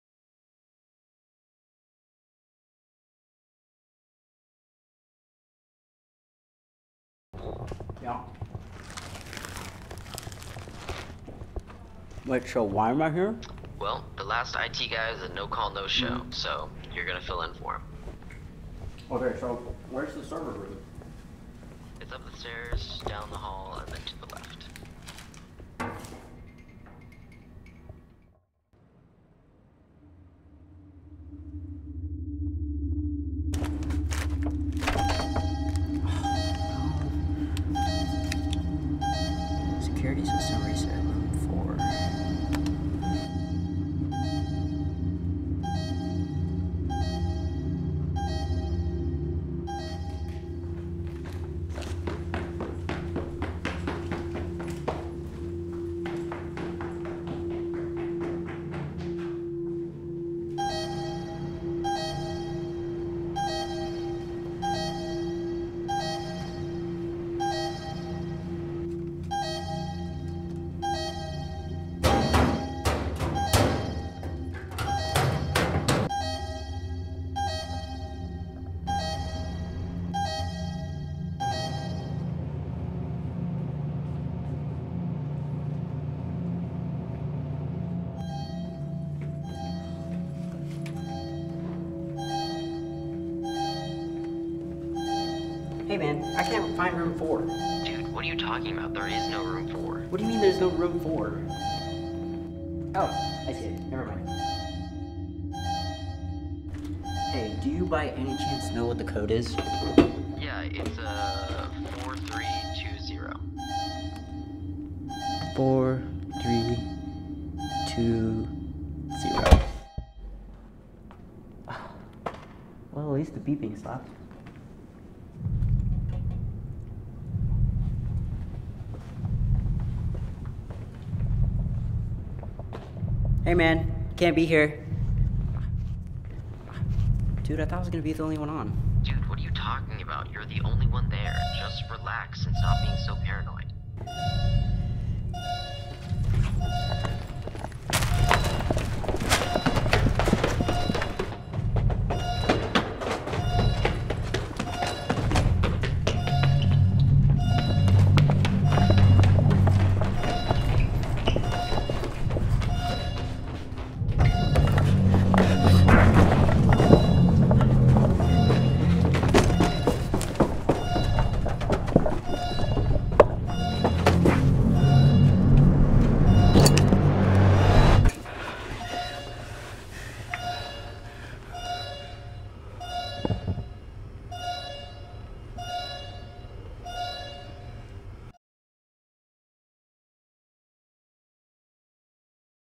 An Uncanny Resemblance
An I.T. employee is filling in for his co-worker that has had several no-call no-shows.